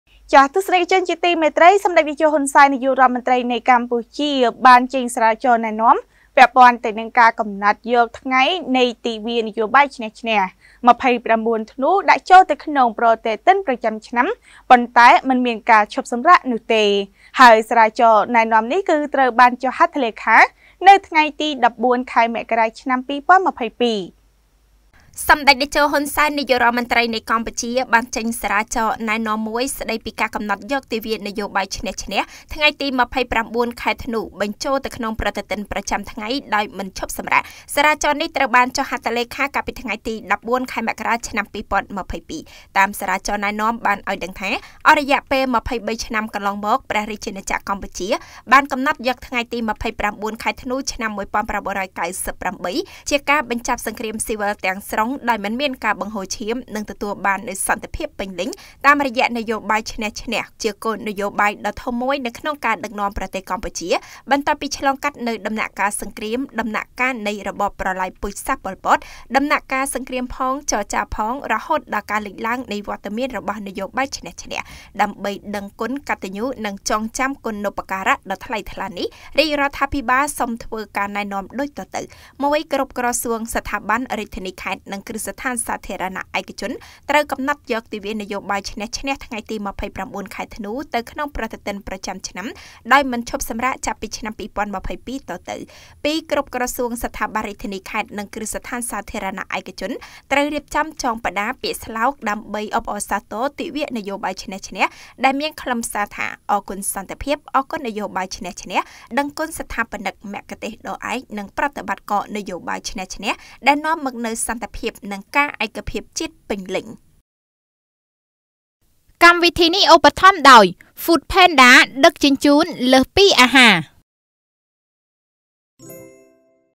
រាជរដ្ឋាភិបាលកម្ពុជា ចេញសារាចរណែនាំបញ្ចូលទិវានយោបាយឈ្នះ ឈ្នះ ២៩ធ្នូ ទៅក្នុងប្រតិទិនប្រចាំឆ្នាំ.
រាជរដ្ឋាភិបាលកម្ពុជា ចេញសារាចរណែនាំបញ្ចូលទិវានយោបាយឈ្នះ ឈ្នះ ២៩ធ្នូ ទៅក្នុងប្រតិទិនប្រចាំឆ្នាំ ដោយមិនឈប់សម្រាក
* រាយការណ៍ព័ត៌មានដោយកញ្ញា ថាន  រស្មី និងកញ្ញា  យ៉ង់ សក្គុណា, ទូរទស្សន៍ព័ត៌មាន Fresh News